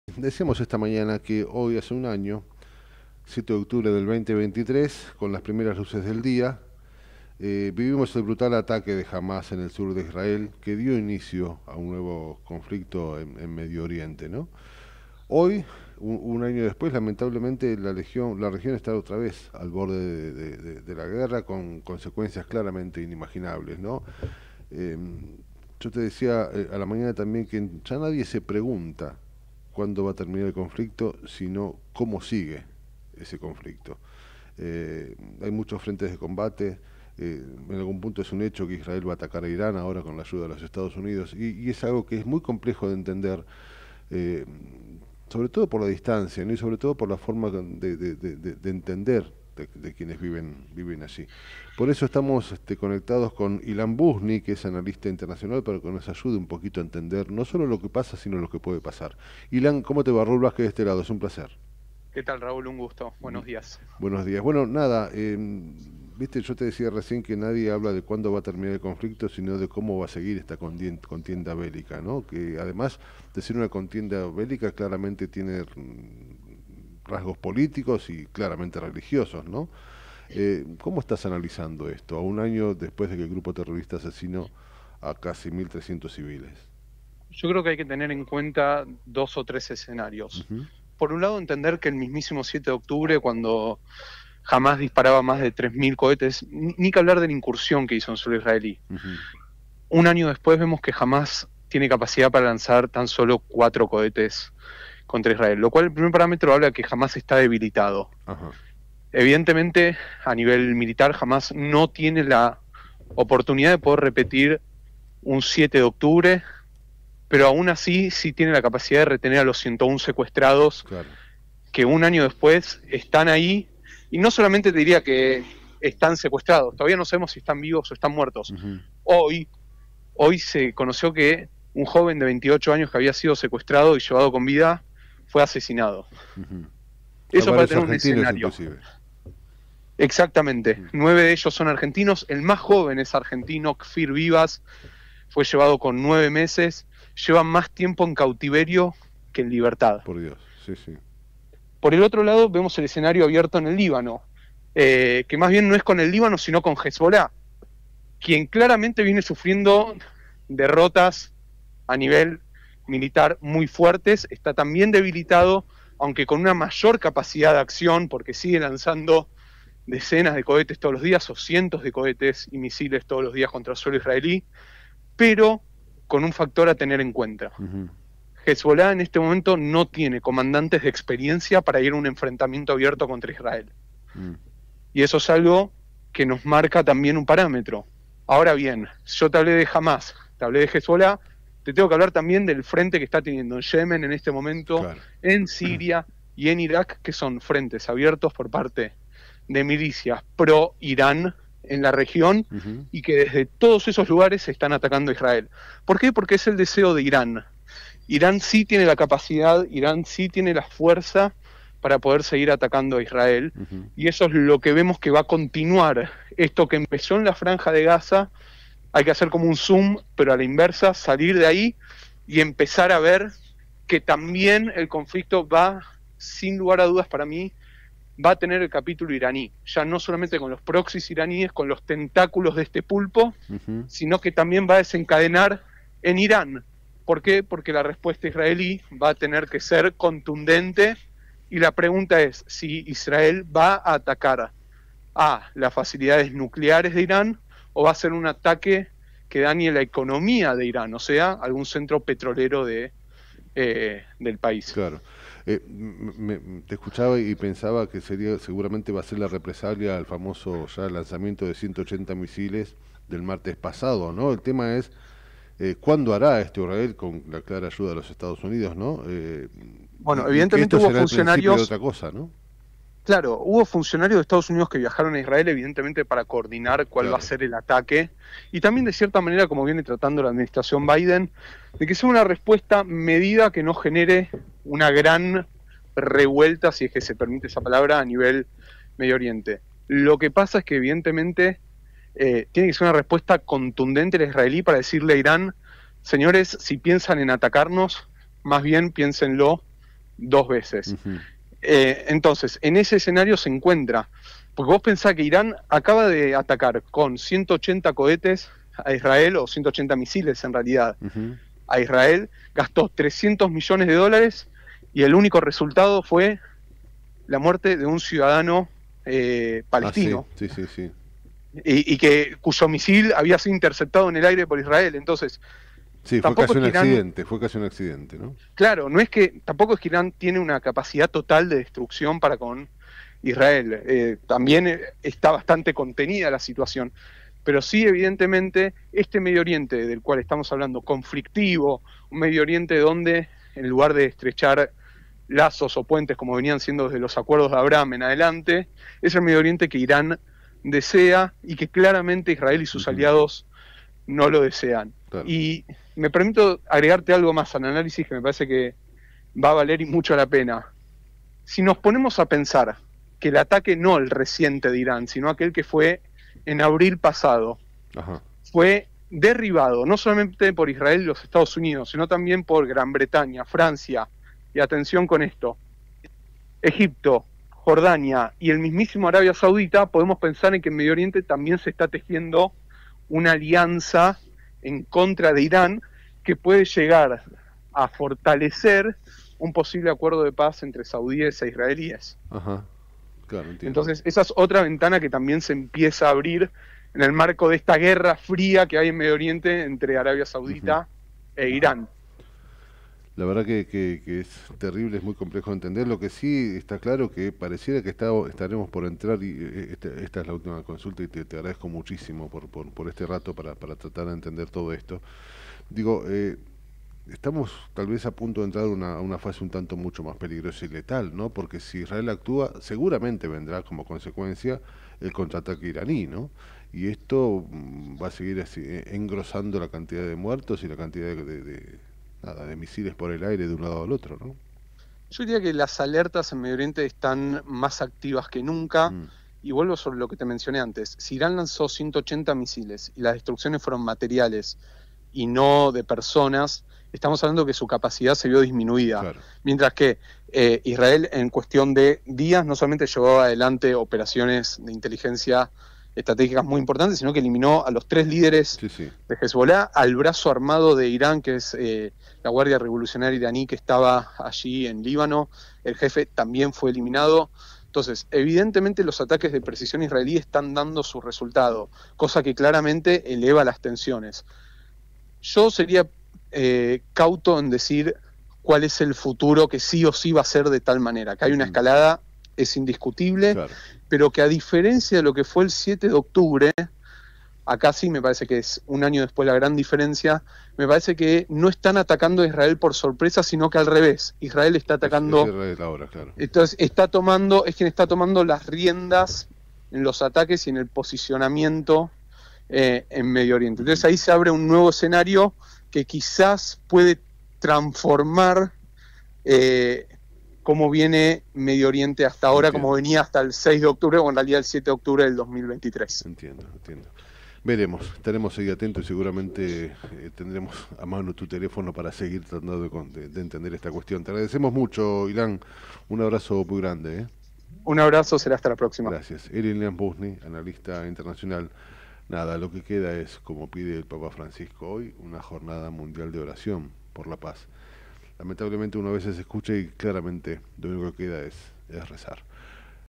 Análisis de la Situación en Medio Oriente: Conflictos y Perspectivas
Ilan Buzny (Analista Internacional) En La Trinchera